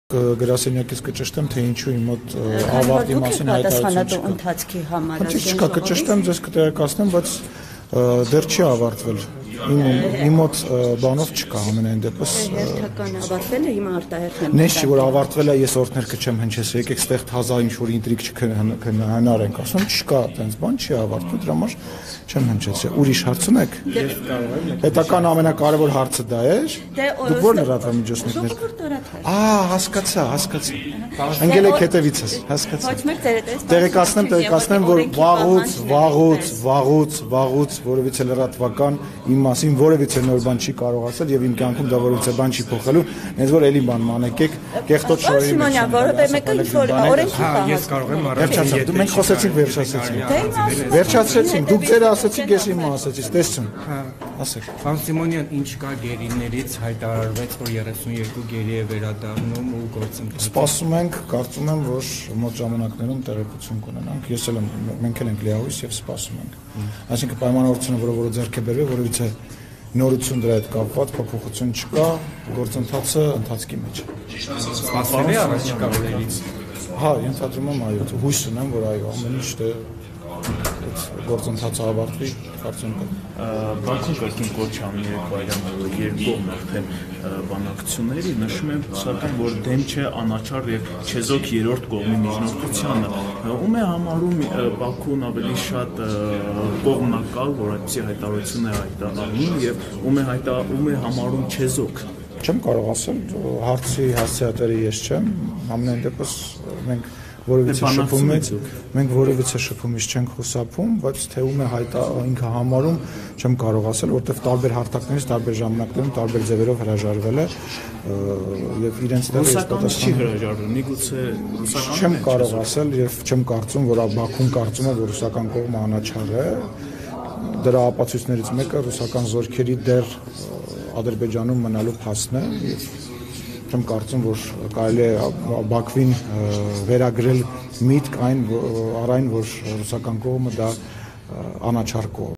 Ալեն Սիմոնյանը՝ գերիների վերադարձի մասին
«Սպասում ենք, կարծում եմ, մոտ ժամանակներում տեղեկություն կունենանք»,- լրագրողների հետ զրույցում անդրադառնալով 32 գերիների վերադարձի մասին հարցին՝ ասել է ԱԺ նախագահ Ալեն Սիմոնյանը։ «Գործընթացն ընթացքի մեջ է»,- նշել է նա։

Օրվա կարևոր լուրերն՝ asekose.am կայքում